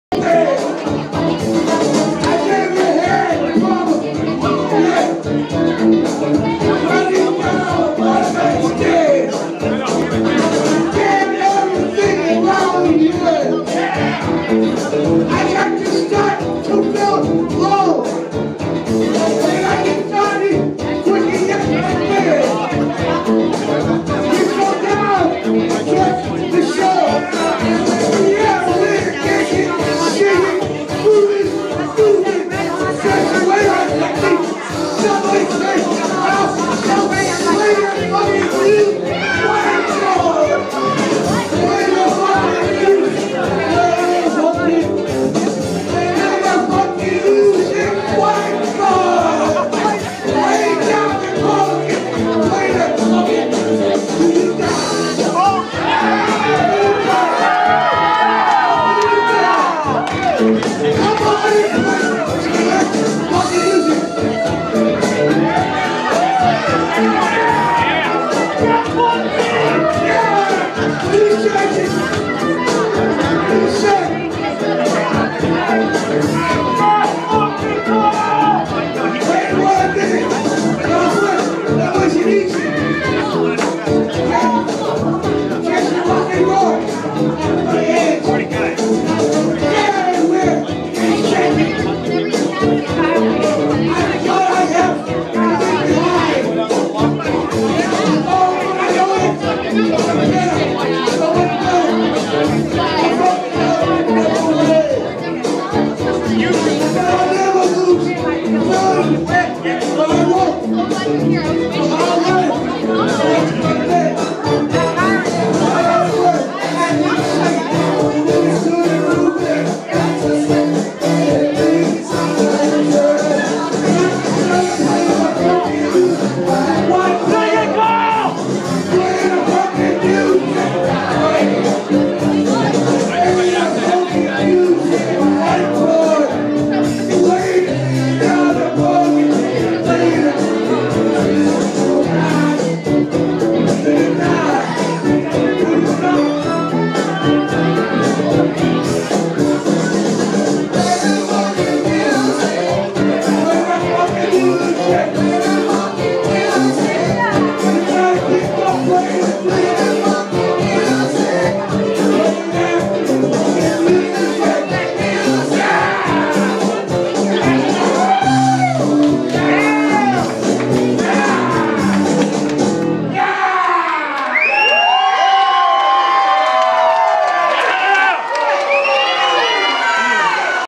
Awesome but horrible karaoke
Horrible singer but can get the fuckin crowd pumped